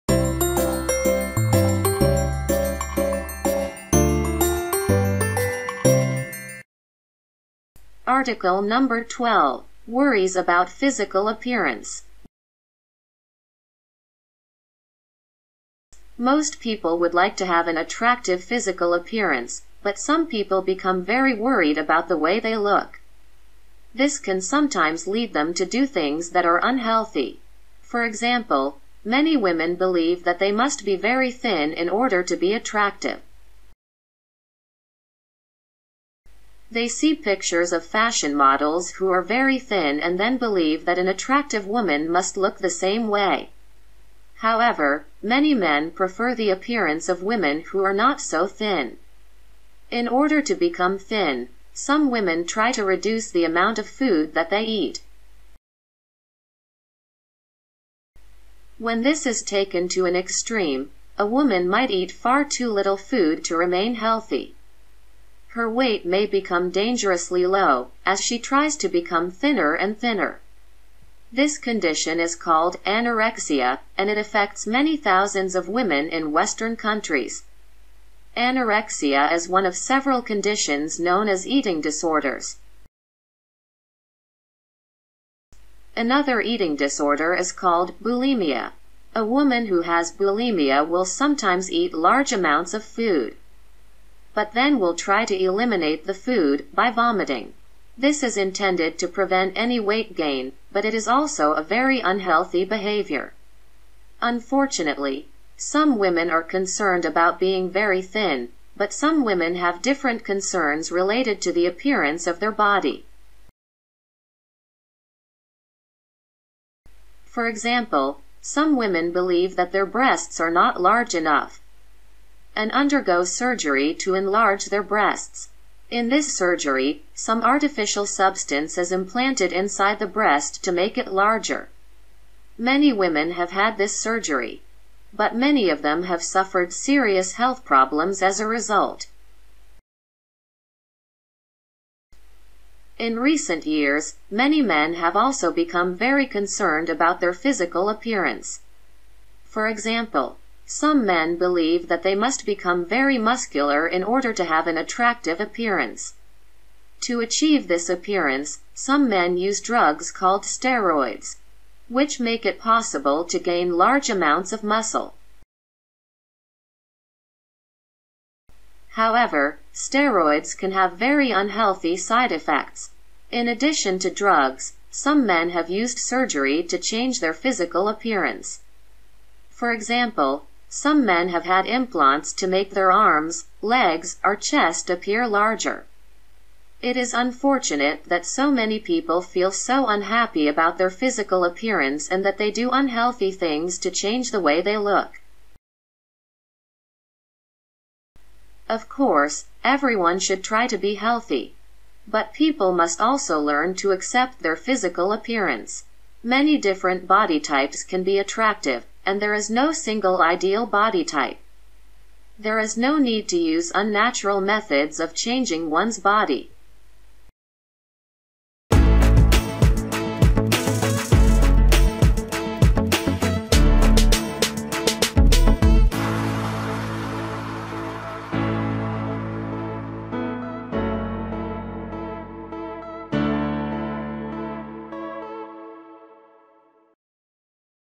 Learn English Practice Reading Listening English Level 3 “Worries About Physical Appearance”
Reading Listening English Level 3 “Worries About Physical Appearance”